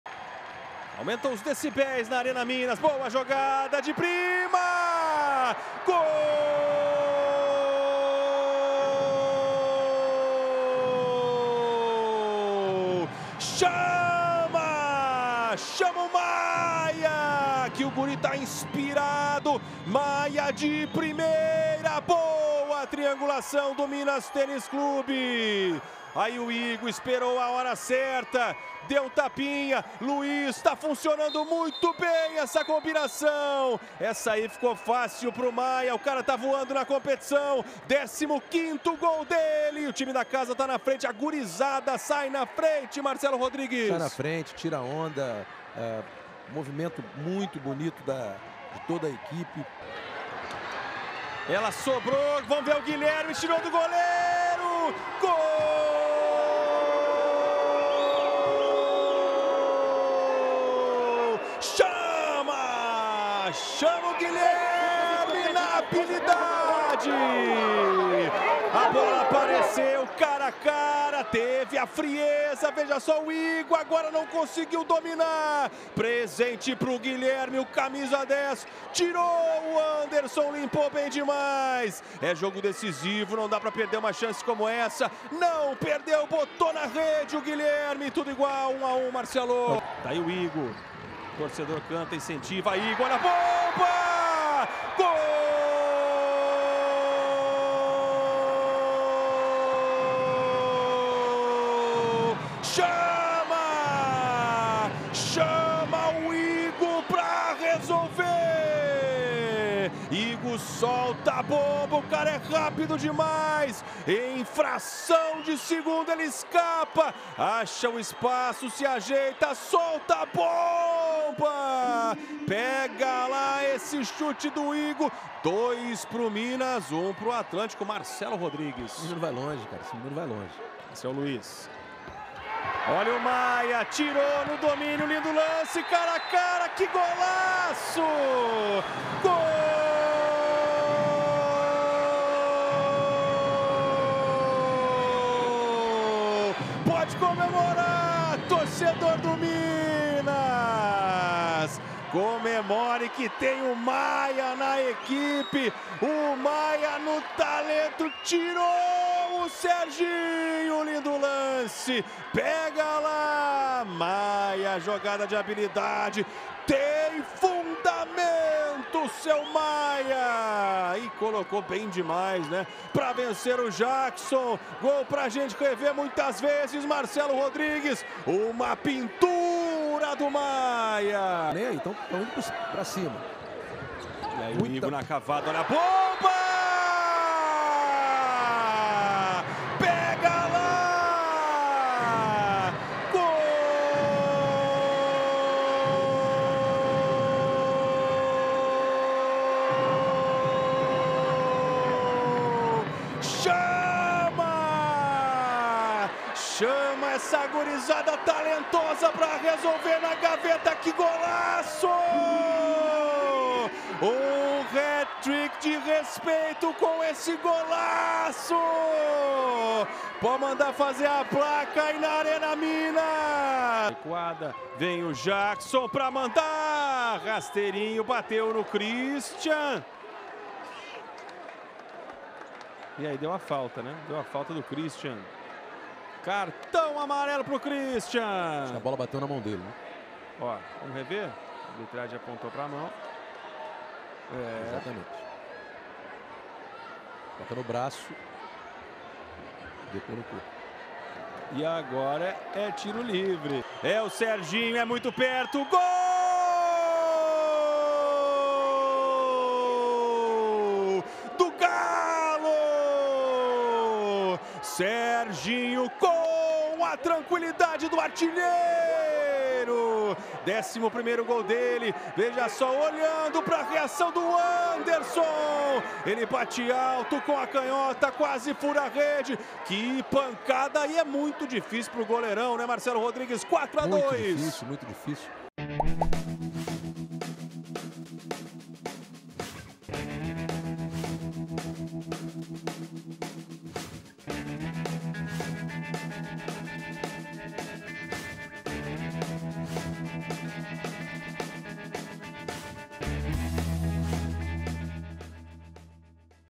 MINAS 4 X 2 ATLÂNTICO | GOLS | LIGA NACIONAL DE FUTSAL | sportv
Minas vence o Atlântico por 4 a 2, pelas quartas de final da Liga Nacional de Futsal 2022.